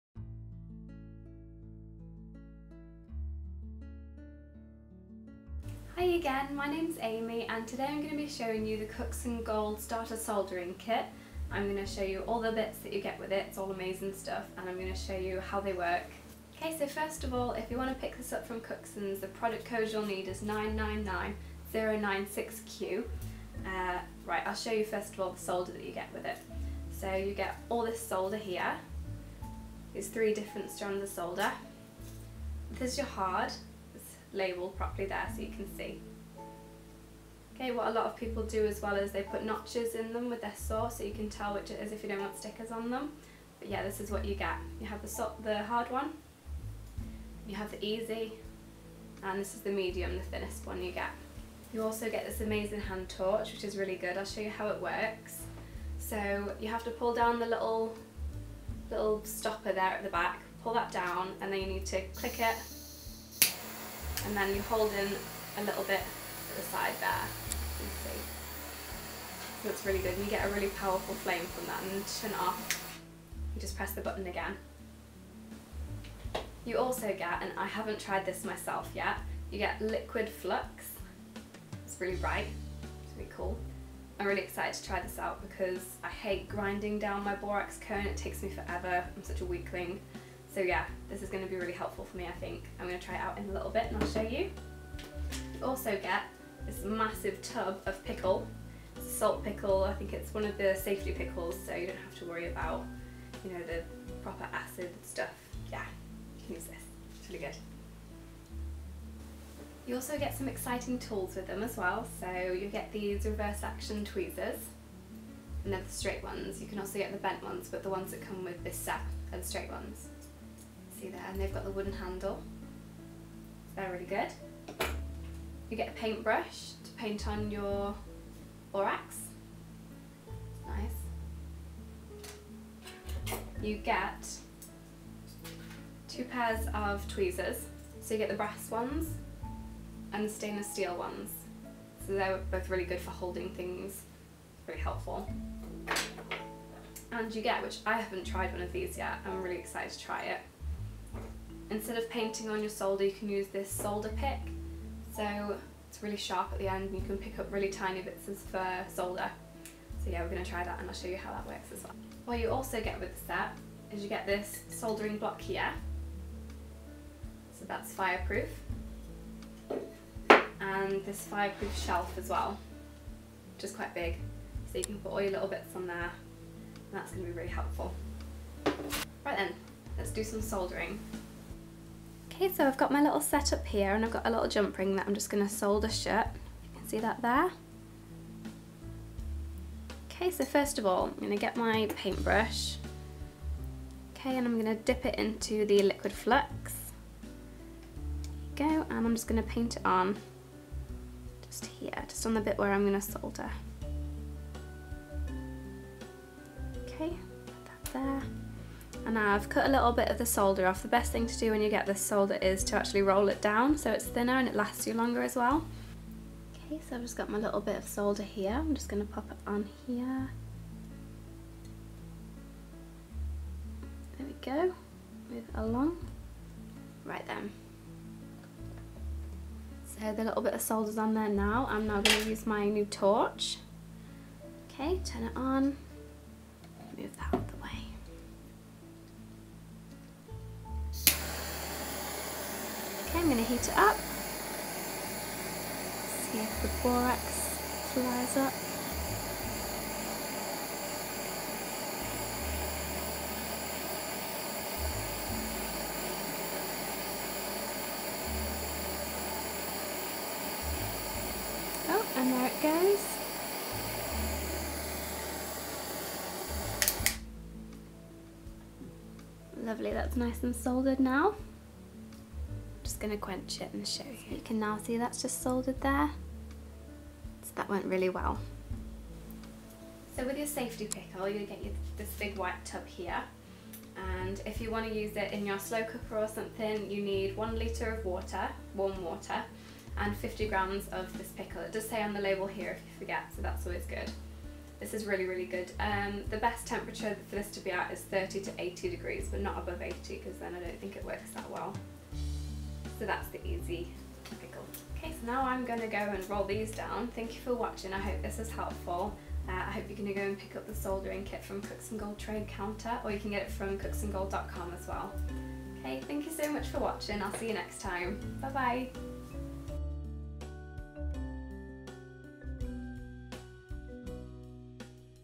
Cooksongold Starter Soldering Kit - Customer Review by Aimee Louise Booth
Cooksongold customer Aimee reviews the Starter Soldering Kit.
This starter soldering kit is ideal either for a home workshop and those starting out in soldering, or those wishing to refresh tired and used soldering tools.

This kit contains a variety of soldering tools and accessories including a hand torch, soldering block, soldering sheet, solder probe, three sets of tweezers, pickling salts, flux and a selection of silver solders.

Aimee reviewed the Starter Soldering Kit as part of Cooksongolds Video Review Team. Want to join, and be in with a chance to get free products to keep?